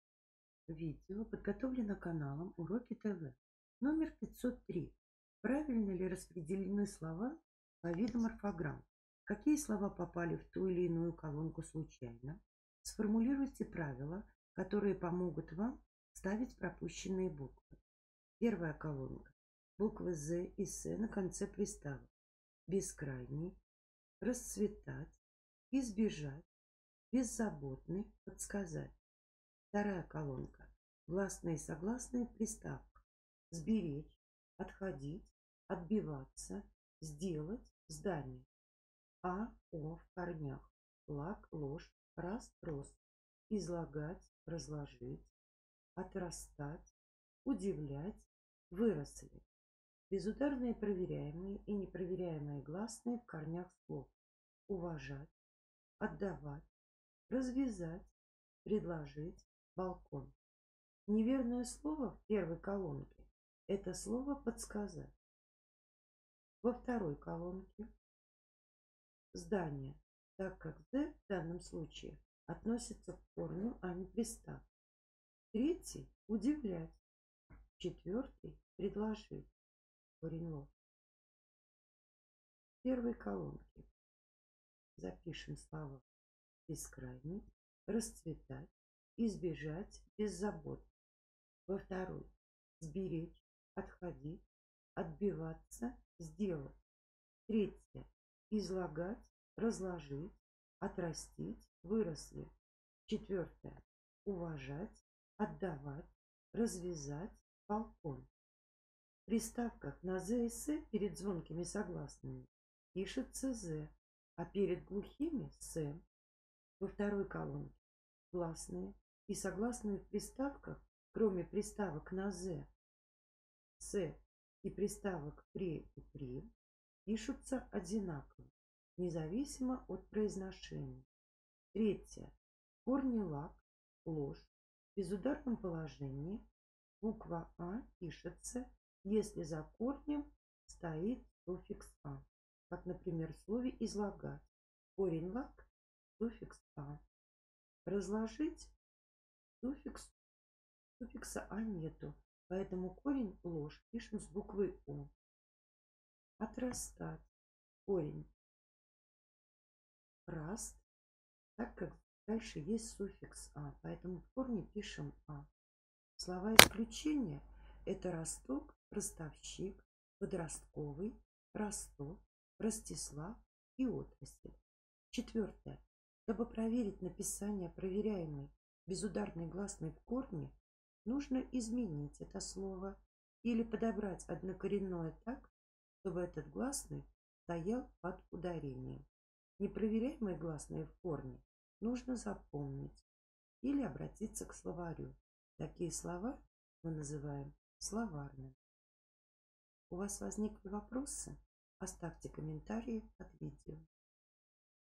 Упражнение №503 — Гдз по русскому языку 5 класс (Ладыженская) 2019 часть 2
В данном номере определяем, правильно ли распределены слова по видам орфограмм, какие слова попали в ту или иную колонку случайно. Сформулируем правила, которые помогут вам вставить пропущенные буквы.